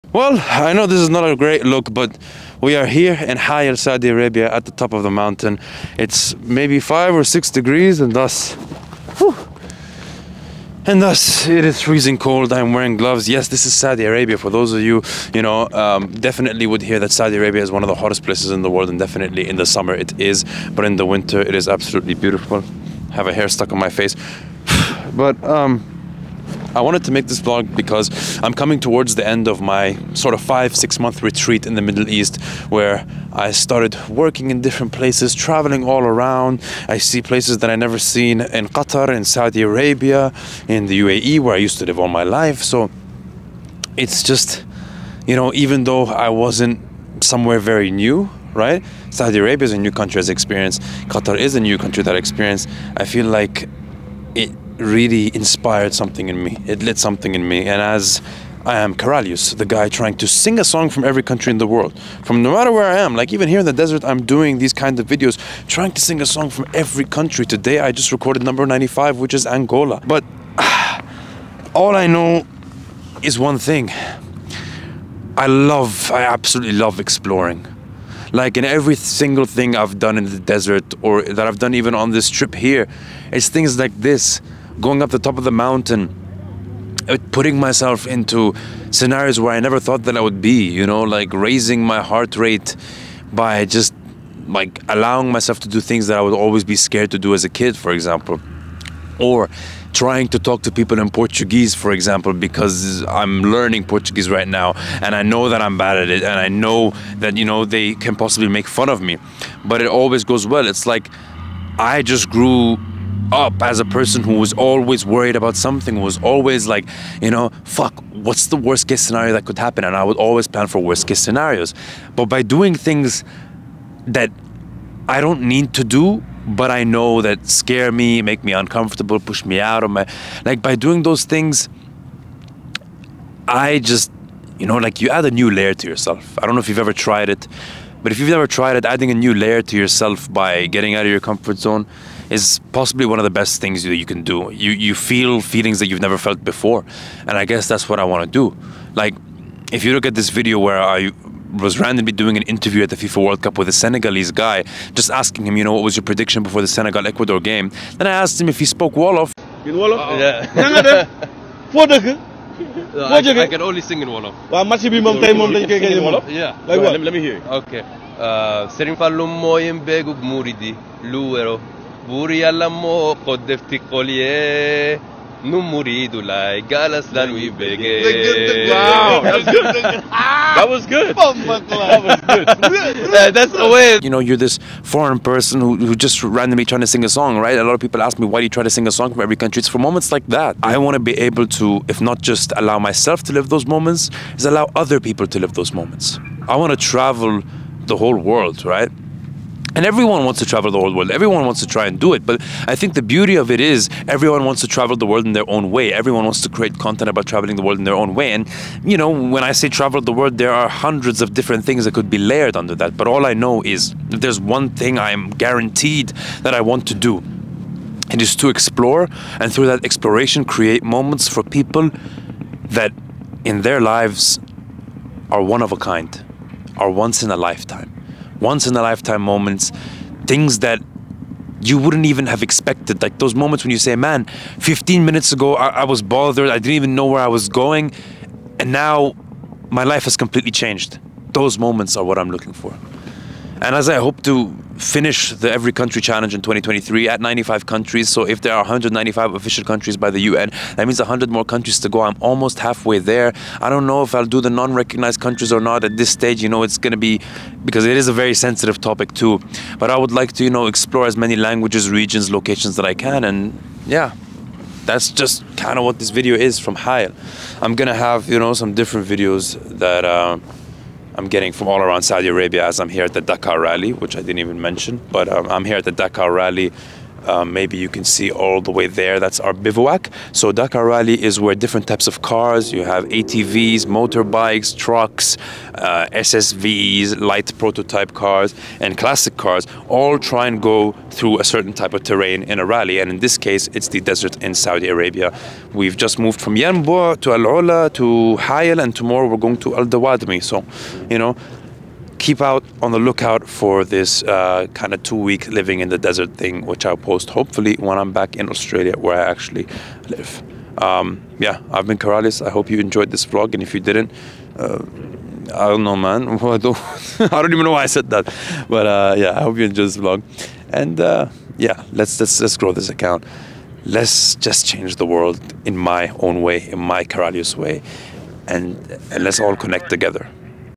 Mountain Reflections: Why I love what I do 🏔
OOOOOFT. Ha'il was cold. But I enjoyed every minute of it despite my body freezing itself out at some points aaaah. I write this knowing that I have more coming on the way and I'm excited to grow this channel. THE WORLD OF KARALIUS, LETS DOOOO THIIISSSS